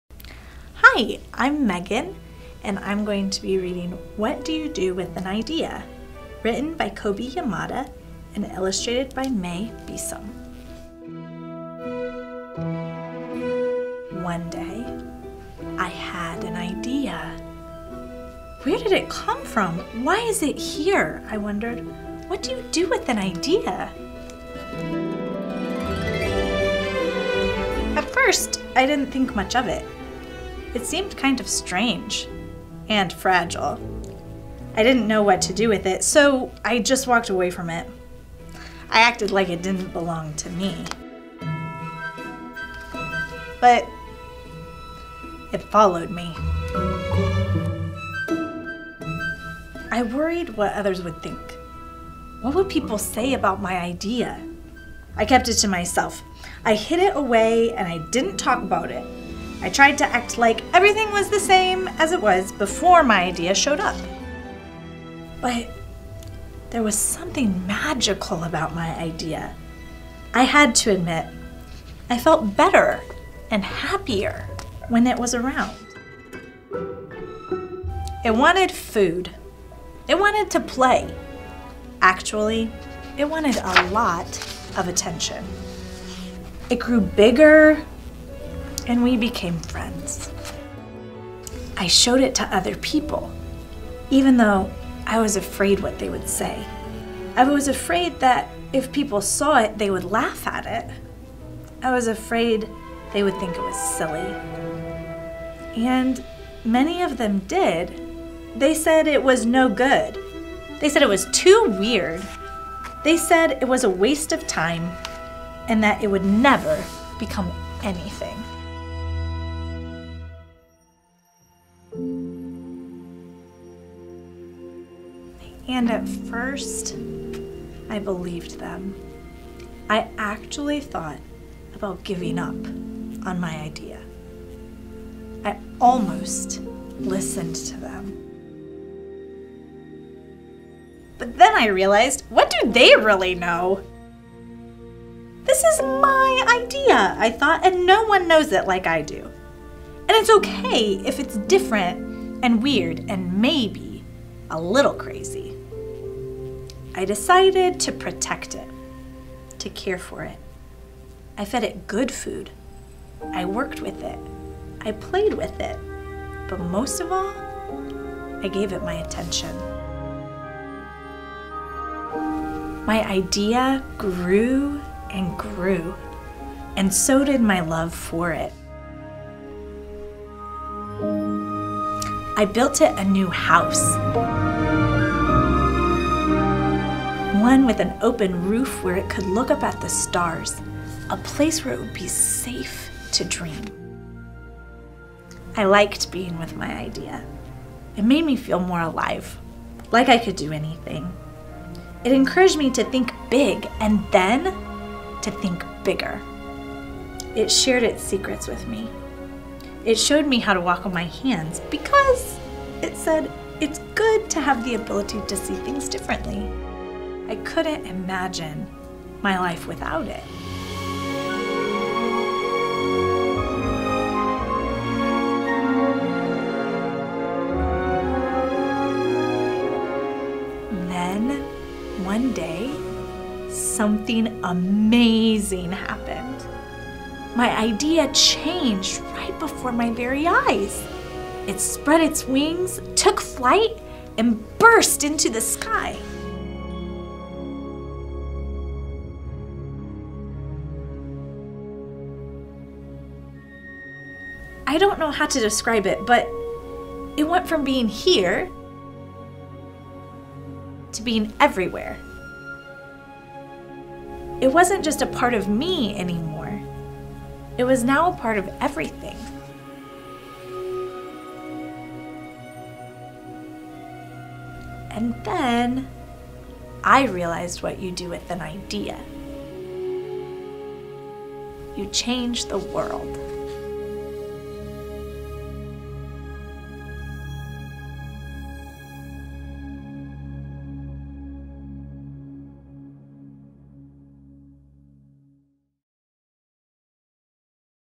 Little Larks 10 - Vision - What do you do with an Idea?
After watching the story with your child, consider the following question for comprehension and discussion:

- What did the boy do to help his idea grow?
- What ideas do you have that are like the one in this book?
- How could you take care of your idea?

Try embodying this story with a yoga story flow by our Well-Being Director, Erica Eyer.

Then ask this critical question: If you could make anything happen, what would it be?

If this video has served you and the people you love, consider making a tax-deductible donation to our work Be well!